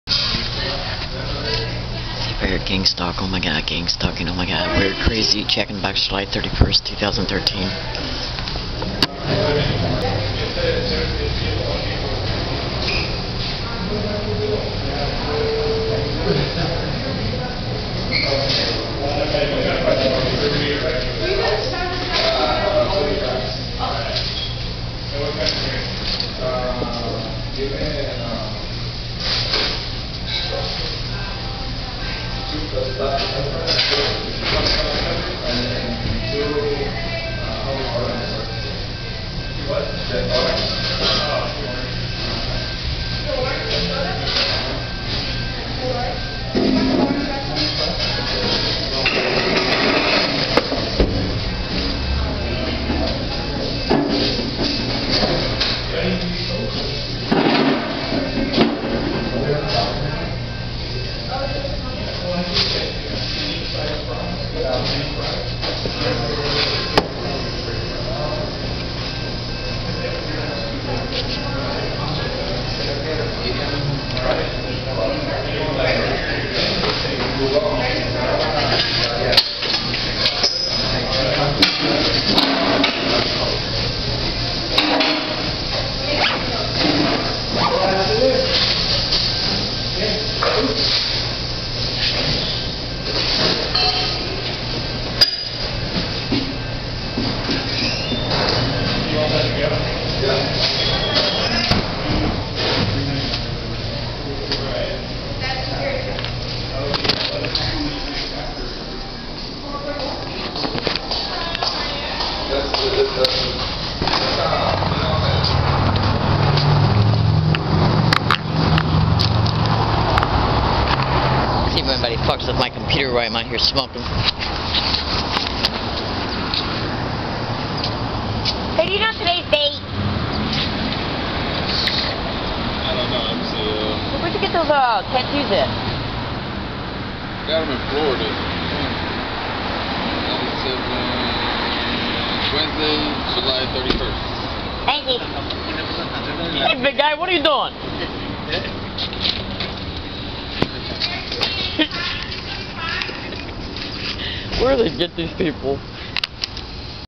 7/31/13 EVIDENCE of Predicted GangStalkinhg Harrasment Prediction Video made in the Morning
Observe People Around her saying GangStalk GangStalking Oh My God
Look at this Video It shows she predicted this in te Morning 30 Miles away A running  recording digital Tape recorder was taping the Entire event as she entered Ordered ate and Numerous Video's show it wide out in the Open Done was tapping the Entire event which shows the stalking of her to put these sensitization methods around her for her to experince